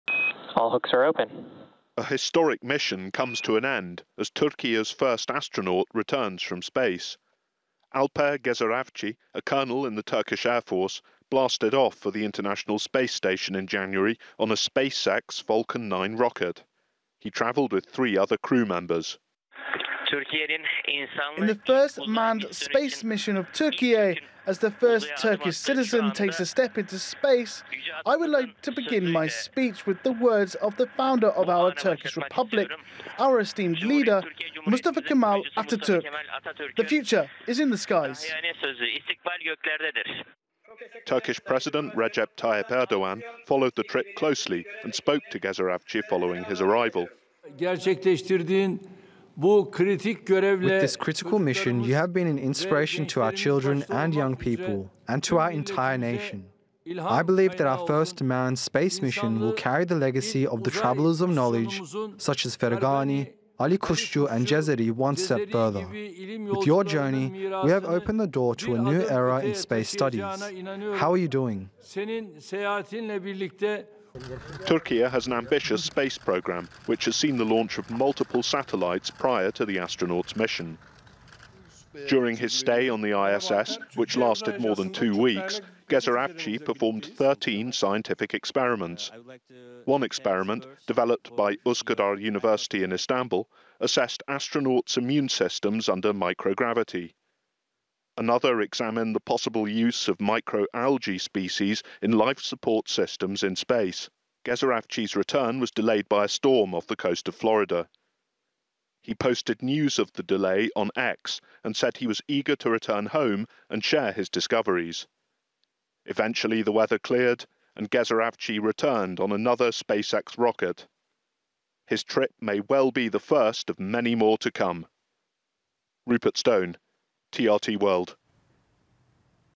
First Turkish astronaut Alper Gezeravci leaves the space station
Türkiye's first astronaut, Alper Gezeravci, left the International Space Station on Wednesday and started his return journey to earth. During the lengthy mission he conducted numerous scientific experiments, as Rupert Stone reports.